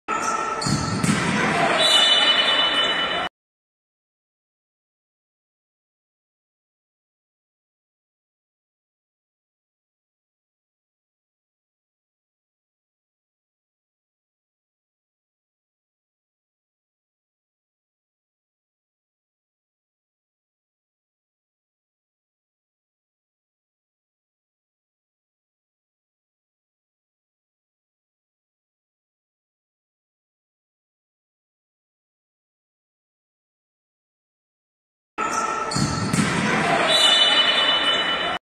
Video 849 Tiro Libre Directo y Amonestación
Dadas las consideraciones, la guardameta comete de manera temeraria una infracción sancionable con tiro libre directo al golpear a la jugadora adversaria Nº 3 amarillo.

«TEMERARIA» es la acción en la que un jugador realiza una acción que entraña daño físico (no necesariamente grave) sin tener en cuenta el riesgo o las consecuencias para su adversario, por lo que deberá ser amonestado.